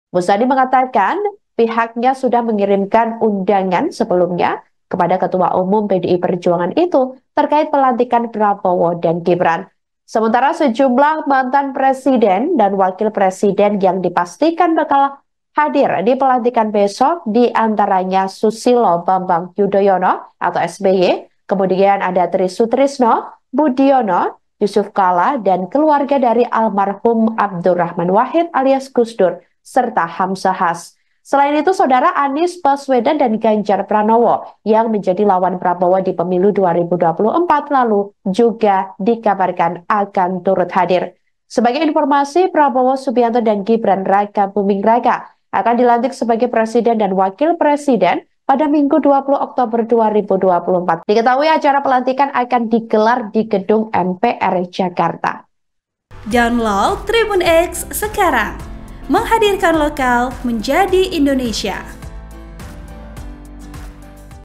MEGAWATI TAK DATANG! SBY HINGGA JUSUF KALLA Hadiri Pelantikan Presiden Prabowo & Wapres Gibran
TRIBUN-MEDAN.COM - Sejumlah mantan presiden & wakil presiden yang
dipastikan bakal hadir dipelantikan Prabowo-Gibran di
antaranya SBY, Try Sutrisno, JK, Keluarga mendiang Presiden Gus Dur & Wapres Hamzah Haz.

Sementara itu, Ketua Umum (Ketum) PDI-P, Megawati Soekarnoputri
dipastikan tidak hadir dalam proses pelantikan Presiden dan Wakil Presiden Terpilih, Prabowo Subianto-Gibran Rakabuming Raka pada Minggu (20/10/2024).

Diketahui, alasan Megawati tidak hadir dalam acara tersebut
karena kurang fit. Hal itu disampaikan oleh Sekjen Partai
Gerindra Ahmad Muzani pada Sabtu (19/10/2024).

Kabar ketidakhadiran Megawati tersebut didapat Muzani dari
Ketua Fraksi PDIP di MPR RI Ahmad Basarah.

Ia mengatakan bahwa Megawati sedang flu. Sehingga
Memutuskan untuk beristirahat.

Editor Video : Chandra